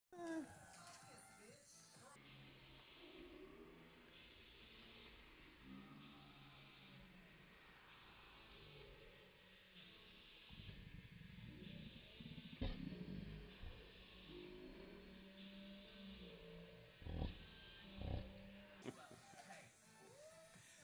DANGEROUSLY CUTE PUP!! Our Italian Greyhuahua works her mommy with a hug.
Cassie deliberately manipulates people by acting cute. Yes, it works.